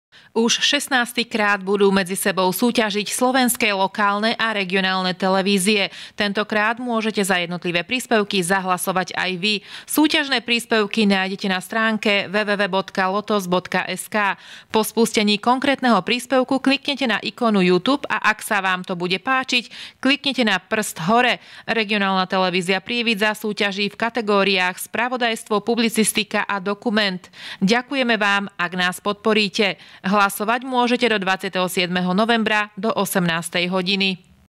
Workshop 2014 – dajte nám svoj hlas
Už 16 - krát budú medzi sebou súťažiť slovenské lokálne a regionálne televízie. Tentokrát môžete za jednotlivé príspevky zahlasovať aj vy. Súťažné príspevky nájdete na stránke Po spustení konkrétneho príspevku kliknete na ikonku youtube a ak sa vám to bude páčiť, kliknete na „prst hore“. Regionálna televízia Prievidza súťaží v kategóriách spravodajstvo, publicistika a dokument. Ďakujeme vám, ak nás podporíte. Hlasovať môžete do 27.novembra do 18.00 hodiny.